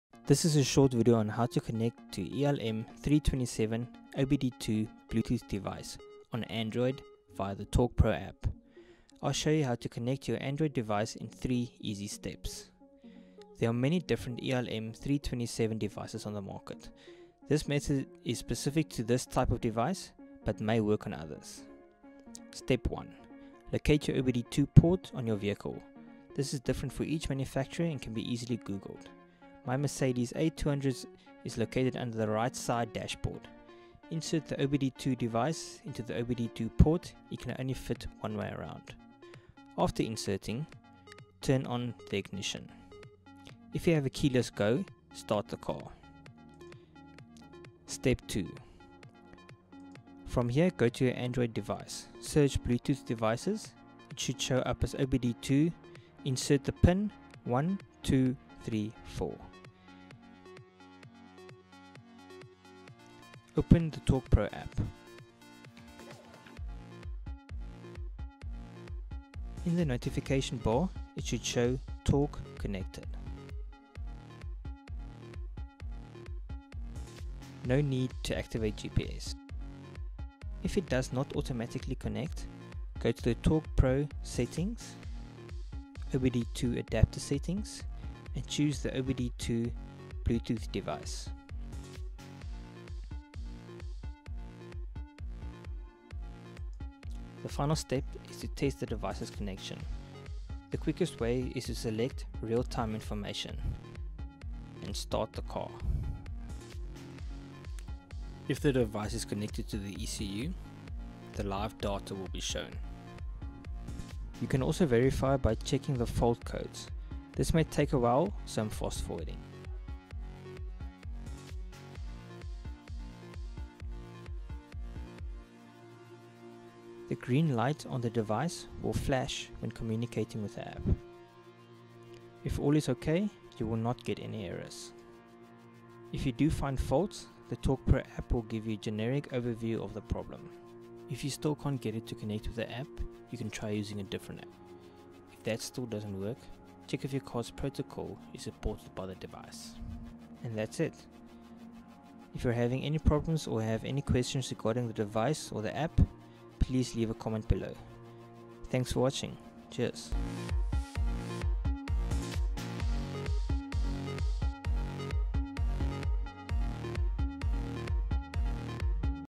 How to connect in 3 easy steps - OBD2 Torque Pro Android
A short video to help you connect your ELM327 OBD2 device to Android Torque Pro App.

Auto Tech Tools

attools.co.za